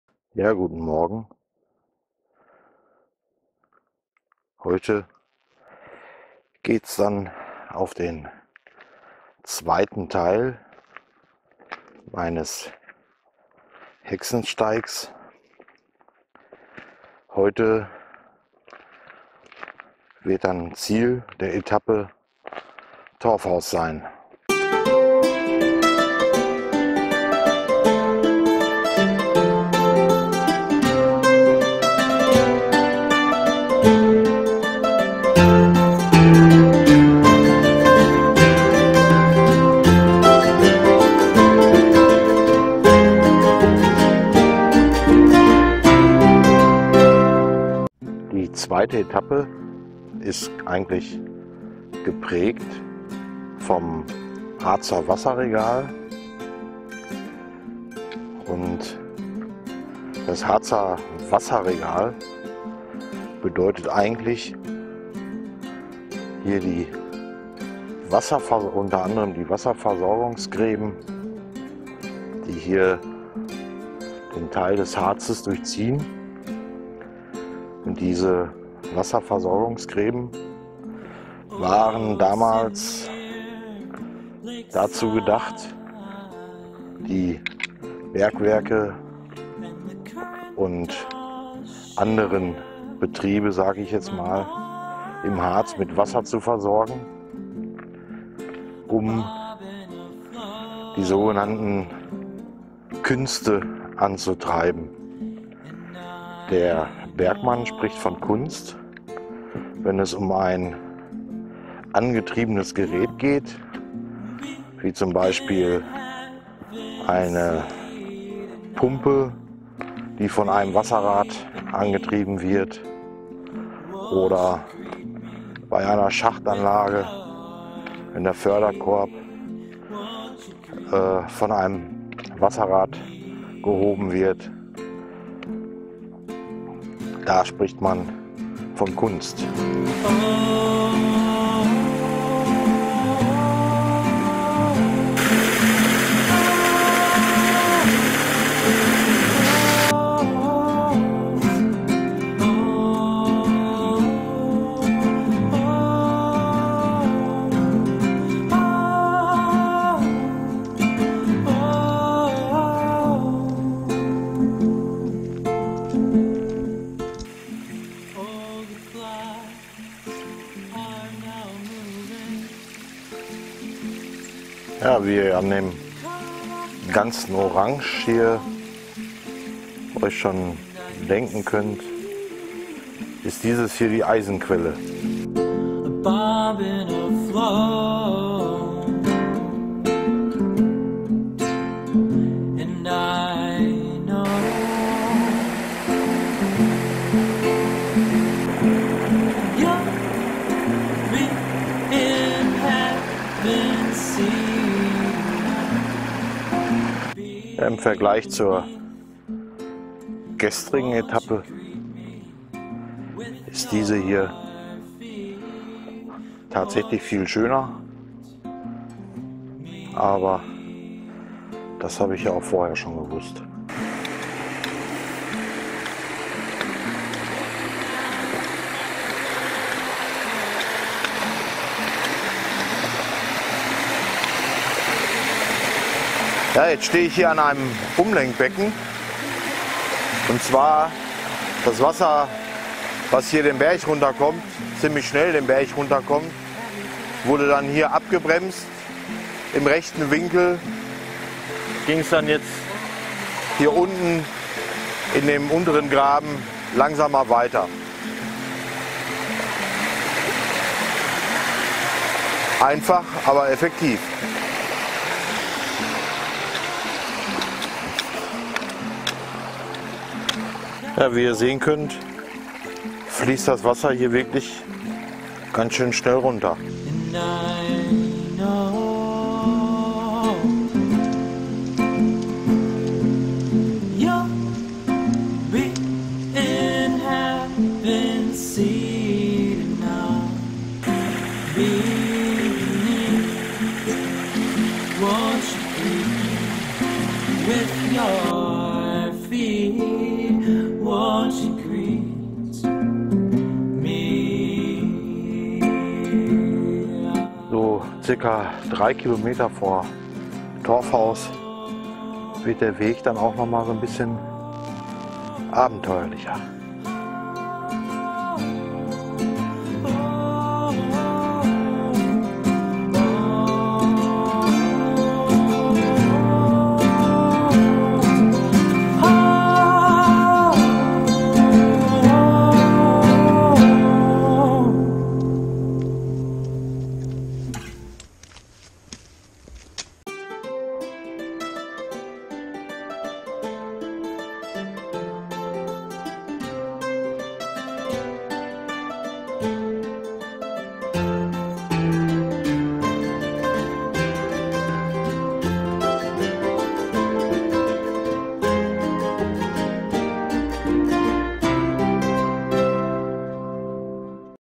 Harzer Hexenstieg Teil 2
Im August 2016 ging ich den Harzer Hexensteig. Dies ist die zweite Etappe ;-)

Meine Hauptkamera
Mein Stativ
Meine Schnittsoftware
Der gute Ton
Rodelink Funkstrecke
Rode Dead Cat
Rode Videomicro